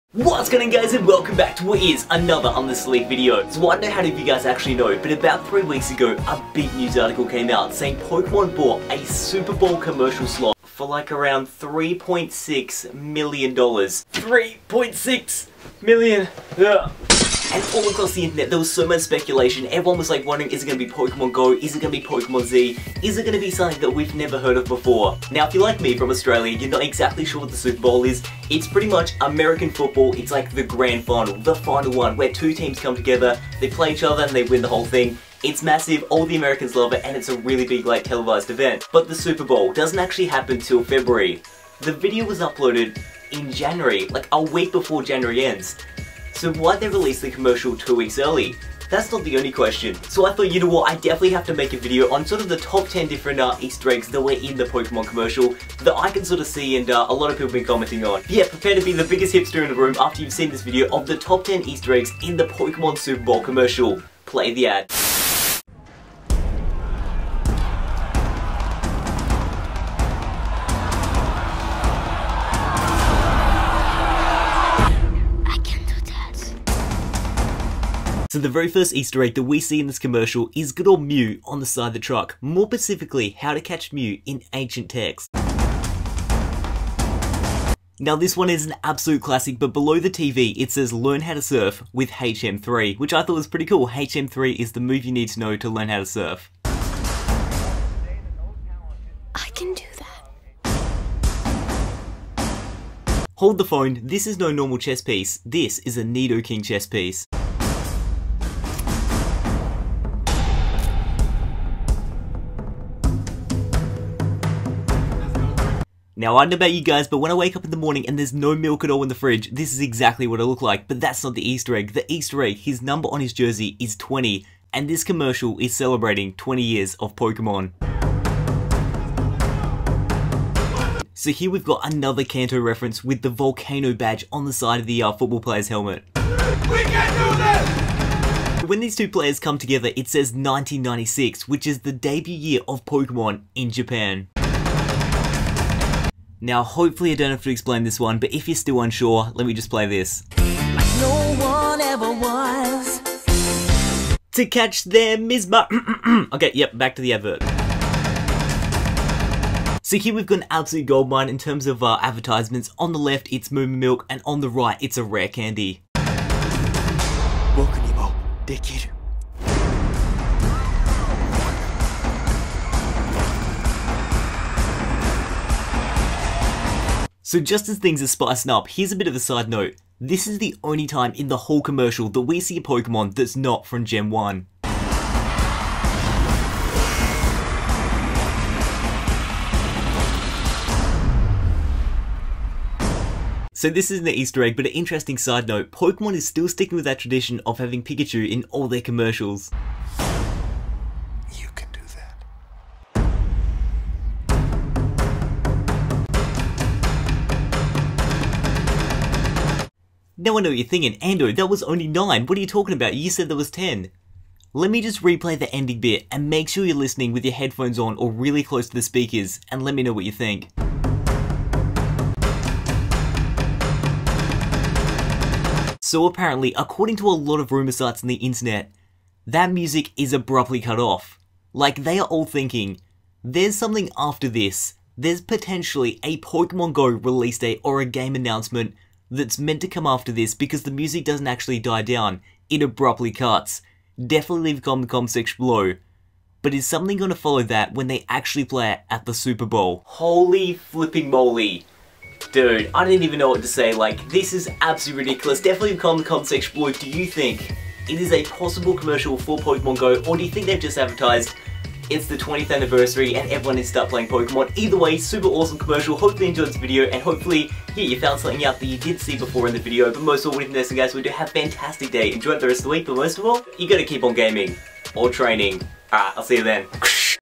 10 Easter Eggs In The Pokemon Super Bowl Commercial!!
The Pokemon Super Bowl Commercial Is finally live!! Today we see how many secrets and hidden Easter Eggs are in the "Train On" 20th Anniversary Pokemon Super Bowl Commercial! We also see if it's maybe a Pokemon Super Bowl Commercial for Pokemon Go...